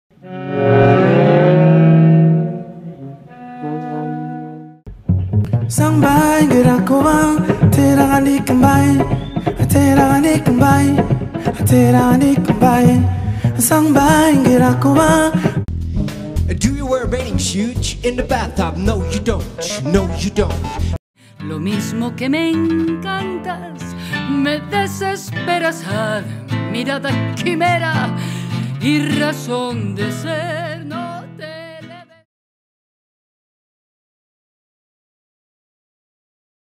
El Trasnoche - FIC 2017
El Festival Internacional Cervantino te invita a sus eventos para trasnochados. Jazz, Tango y música para disfrutar al filo de la madrugada.